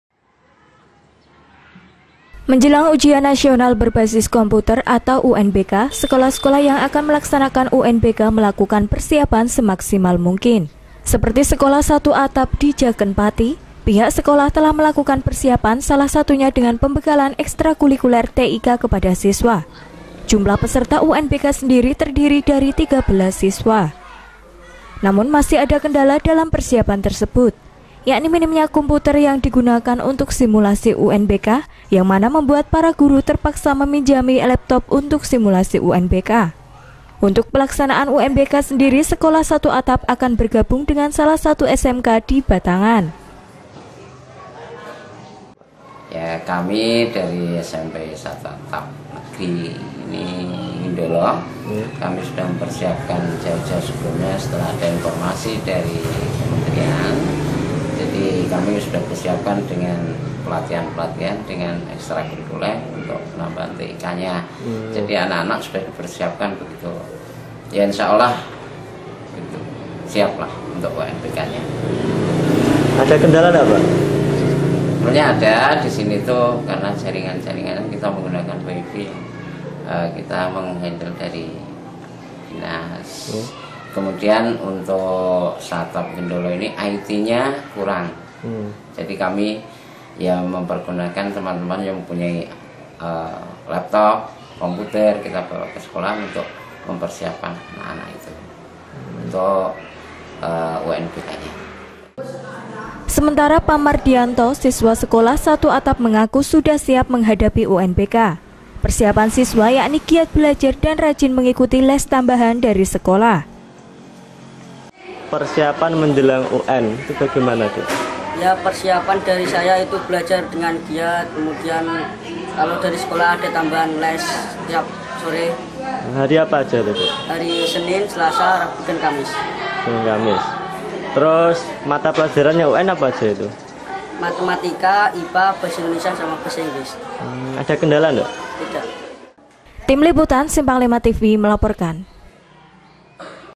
PERSIAPAN SEKOLAH SATU ATAP MENJELANG UNBK
JUDUL  : JELANG UNBK
SUB JUDUL 1: PERSIAPAN SEKOLAH SATU ATAP MENJELANG UNBK
SUB JUDUL 2: KENDALA SARANA PRASARANA JELANG UNBK
LOKASI  : JAKEN-PATI

LEAD  :

MENJELANG UJIAN NASIONAL BERBASIS KOMPUTER/ SEKOLAH SATU ATAP MELAKUKAN PERSIAPAN DENGAN MASKSIMAL// NAMUN MASIH ADA KENDALA/ YAKNI TERBATASNYA KOMPUTER DAN JARINGAN AKSES INTERNET//

ROLL PKG  :

MENJELANG UJIAN NASIONAL BERBASIS KOMPUTER ATAU UNBK// SEKOLAH-SEKOLAH YANG AKAN MELAKSANAKAN UNBK MELAKUKAN PERSIAPAN SEMAKSIMAL MUNGKIN//

SEPERTI SEKOLAH SATU ATAP DI JAKEN/ PATI/ PIHAK SEKOLAH TELAH MELAKUKAN PERSIAPAN SALAH SATUNYA DENGAN PEMBEKALAN EKSTRAKULIKULER TIK KEPADA SISWA// JUMLAH PESERTA UNBK SENDIRI/ TERDIRI DARI 13 SISWA//

NAMUN MASIH ADA KENDALA DALAM PERSIAPAN TERSEBUT/ YAKNI MINIMNYA KOMPUTER YANG DIGUNAKAN UNTUK SIMULASI UNBK// YANG MANA MEMBUAT PARA GURU TERPAKSA MEMINJAMKAN LAPTOP UNTUK SIMULASI UNBK// UNTUK PELAKSANAN UNBK SENDIRI/ SEKOLAH SATU ATAP AKAN BERGABUNG DENGAN SALAH SATU SMK DI BATANGAN//

WWC  : JUWARI/ KEPSEK SMP SATU ATAP JAKEN //

SEMENTARA PAMARDIANTO/ SISWA SEKOLAH SATU ATAP MENGAKU/ SUDAH SIAP MENGHADAPI UNBK// PERSIAPAN SISWA YAKNI GIAT BELAJAR/ DAN RAJIN MENGIKUTI LES TAMBAHAN DARI SEKOLAH//

WWC  : PAMARDIANTO/ SISWA KELAS 9 SMP SATU ATAP//

TIM LIPUTAN/ SIMPANG5TV/ MELAPORKAN//